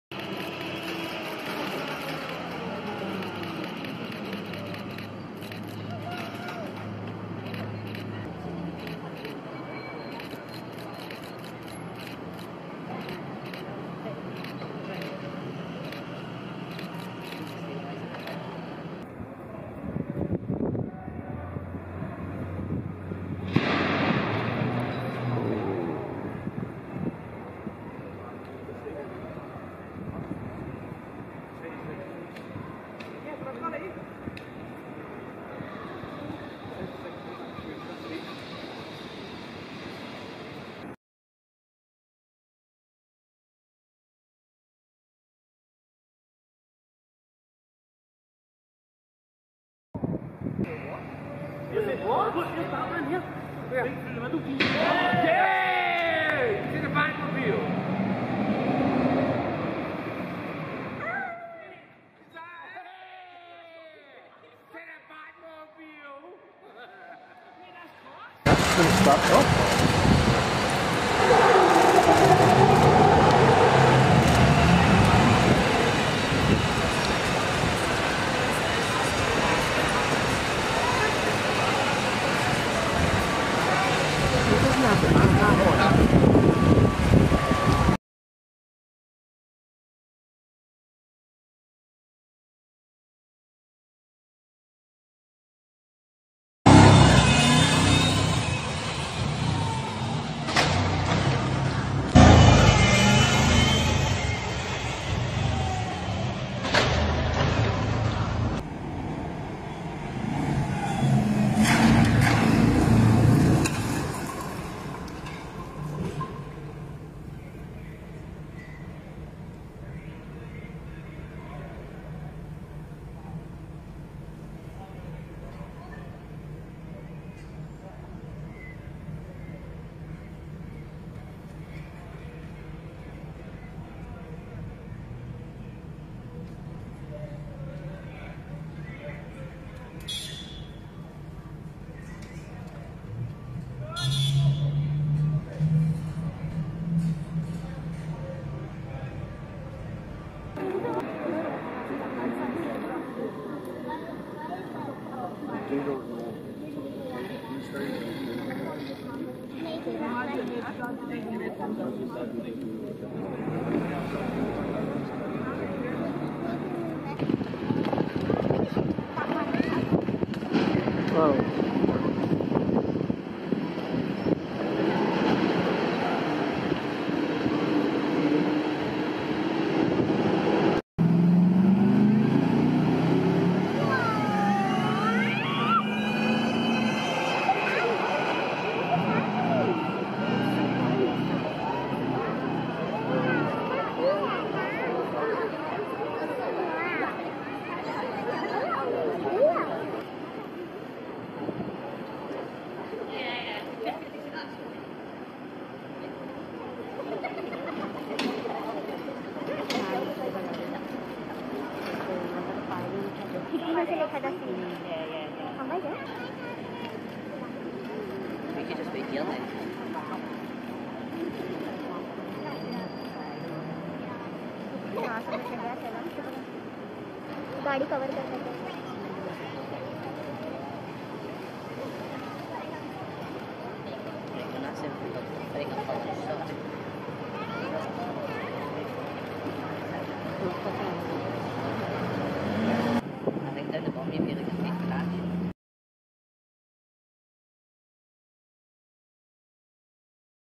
BATMAN BATCYCLE ACCIDENT ON SET - The FLASH MOVIE (2022)
Ben Affleck Batman Batcycle had two crashes on the set of The Flash movie.
The Flash is currently filming in Glasgow,